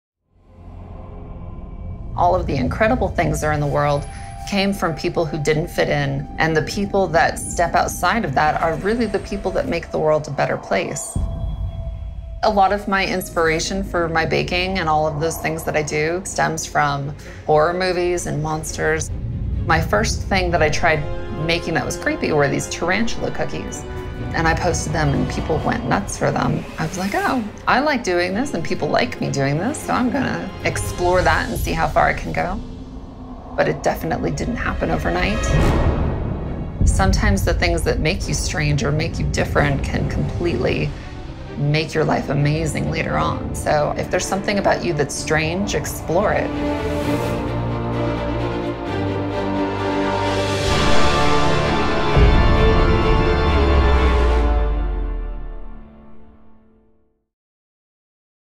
Peculiar Redefined -Christine McConnell
20th Century Fox Came to my home to film me working on a project for the new film Miss Peregrine's Home for Peculiar Children :D